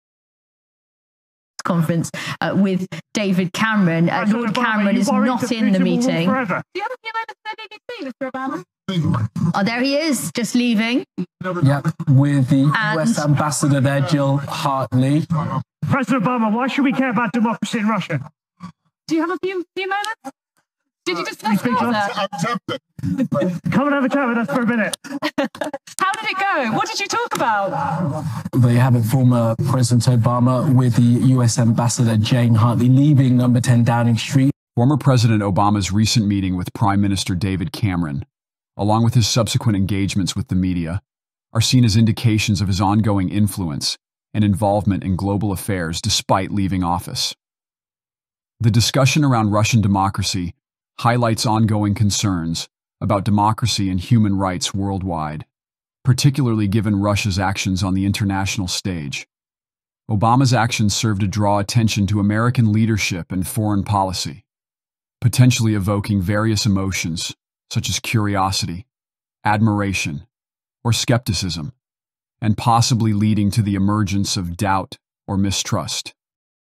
Obama and US Ambassador, Leave 10 Downing St
Witness the surprising departure of Barack Obama from 10 Downing Street, accompanied by US Ambassador Jane Hartley! Stay updated with the latest news as we delve into the details of this unexpected encounter.